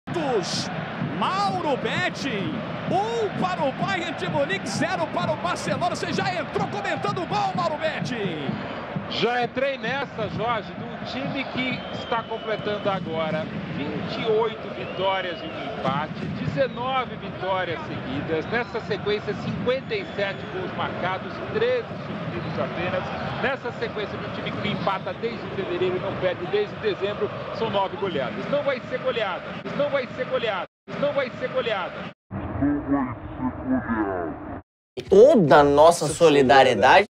“Não vai ser goleada”-BETING,Mauro(2020)
Declaração sem querer do jornalista Mauro Beting do Esporte Interativo durante o jogo da Champions League, Barcelona 2 x 8 Bayern de Munique